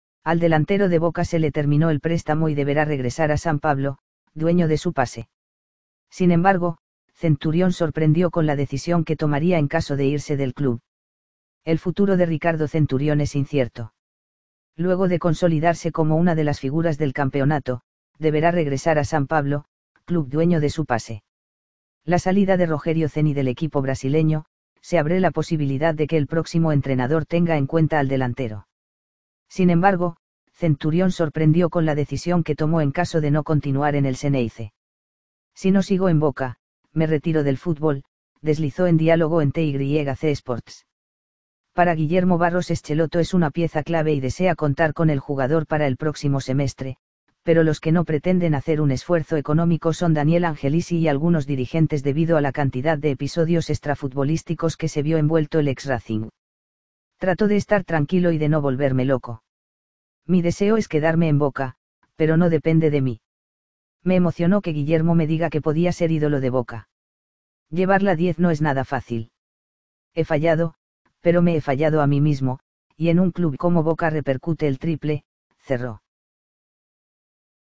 La Dura advertencia Que Lanzo Centurión!
Al delantero de Boca se le terminó el préstamo y deberá regresar a San Pablo, dueño de su pase. Sin embargo, Centurión sorprendió con la decisión que tomaría en caso de irse del club.

El futuro de Ricardo Centurión es incierto. Luego de consolidarse como una de las figuras del campeonato, deberá regresar a San Pablo, club dueño de su pase. La salida de Rogerio Ceni del equipo brasileño, se habré la posibilidad de que el próximo entrenador tenga en cuenta al delantero.

Sin embargo, Centurión sorprendió con la decisión que tomó en caso de no continuar en el Xeneize. "Si no sigo en Boca, me retiro del fútbol", deslizó en diálogo en TyC Sports. Para Guillermo Barros Schelotto es una pieza clave y desea contar con el jugador para el próximo semestre, pero los que no pretenden hacer un esfuerzo económico son Daniel Angelici y algunos dirigentes debido a la cantidad de episodios extrafutbolísticos que se vio envuelto el ex Racing.

"Trato de estar tranquilo y de no volverme loco. Mi deseo es quedarme en Boca, pero no depende de mí. "Me emocionó que Guillermo me diga que podía ser ídolo de Boca. Llevar la 10 no es nada fácil. He fallado, pero me he fallado a mí mismo, y en un club como Boca repercute el triple", cerró.